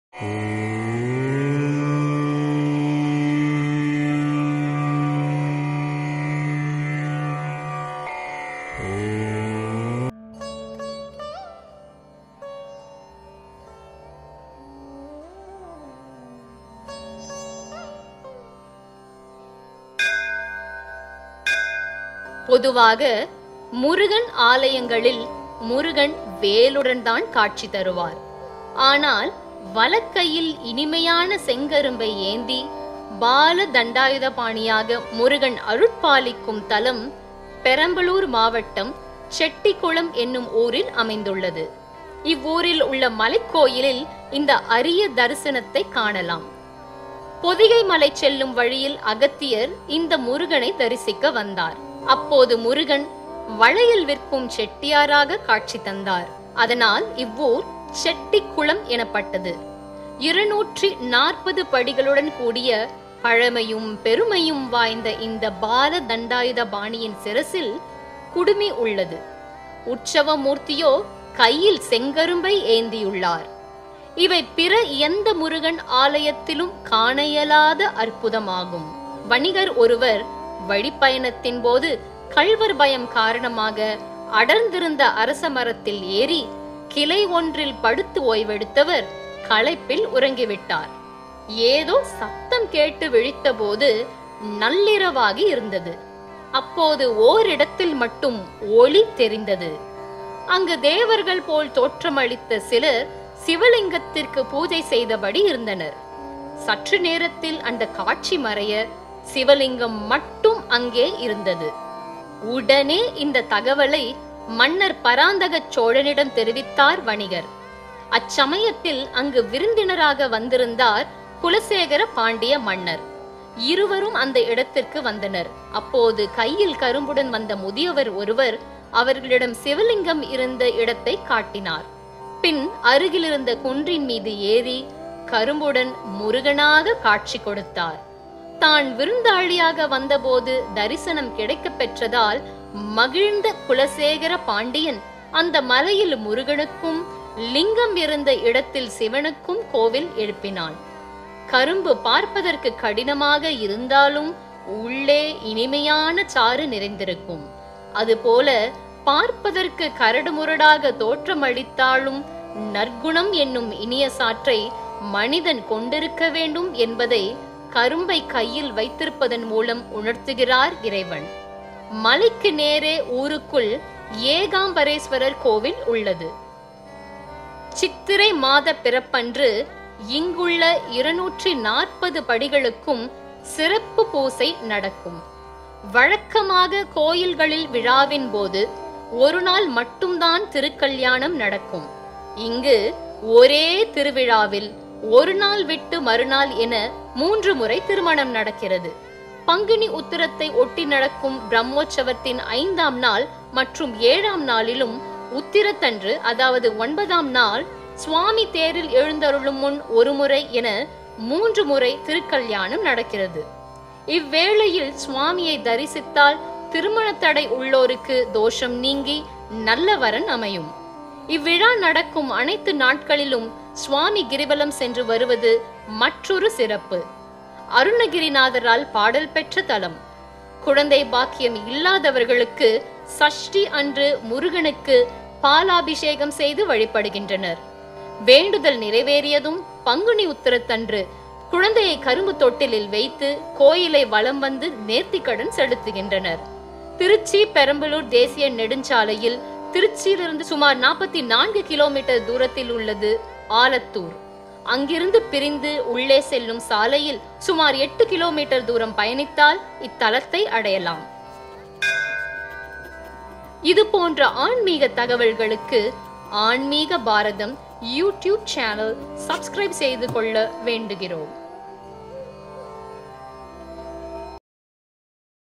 கையில் கரும்பு..முகத்தில் குறும்பு! செட்டிகுளம் முருகன் வரலாறு!
பொதுவாக முருகன் ஆலயங்களில் முருகன் வேலுடன்தான் காட்சி தருவார். ஆனால் வலக்கையில் இனிமையான செங்கரும்பை ஏந்தி பாலதண்டாயுதபாணியாக முருகன் அருட்பாலிக்கும் தலம், பெரம்பலூர் மாவட்டம் செட்டிக்குளம் எனும் ஊரில் அமைந்துள்ளது . இவ்வூரில் உள்ள மலைக்கோயிலில் இந்த அரிய தரிசனத்தைக் காணலாம்.